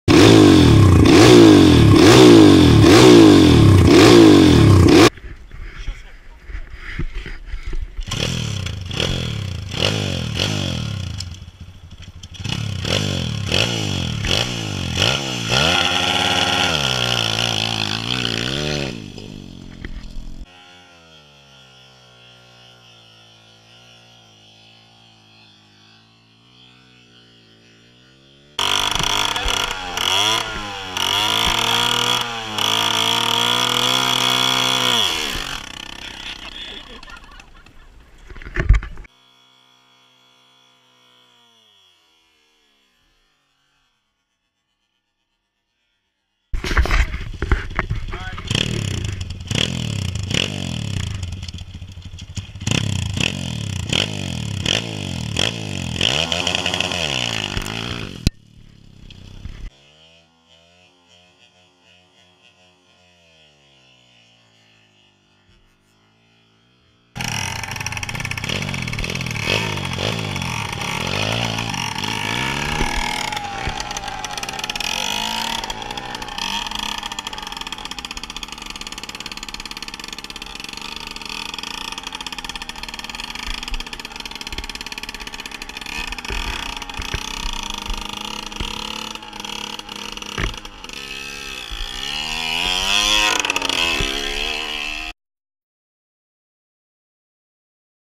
trying race starts
trying race starts on the demon x 140 and the Suzuki tx125